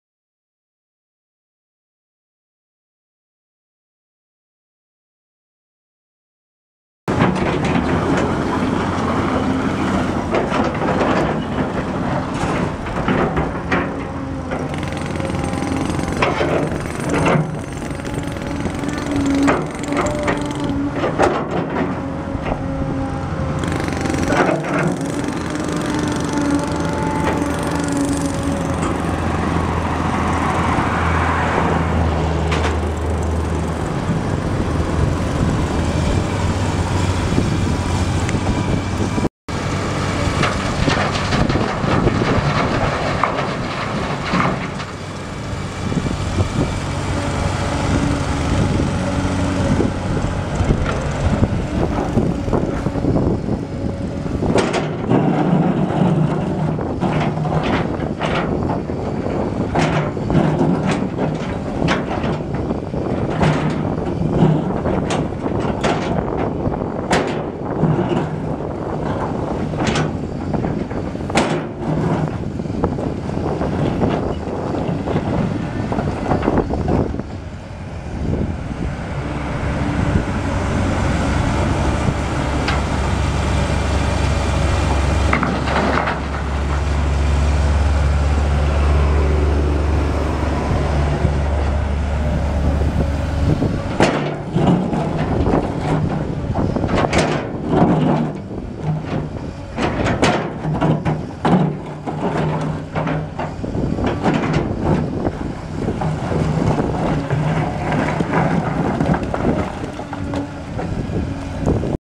2003 Caterpillar 325CL excavator for sale | sold at auction May 31, 2013
2003 Caterpillar 325CL excavator
6,977 hours on meter
Caterpillar 3126 7.2L six cylinder turbo diesel engine
309 HP
Suspension seat
Straight travel
Work lights
Pattern changer
54" bucket with six teeth
31"W triple grouser track pads
63,100 lbs. base operating weight
Serial CAT0325CTBFE00551

It was located in Afton, OK, and we sold it to the highest bidder for $66,000.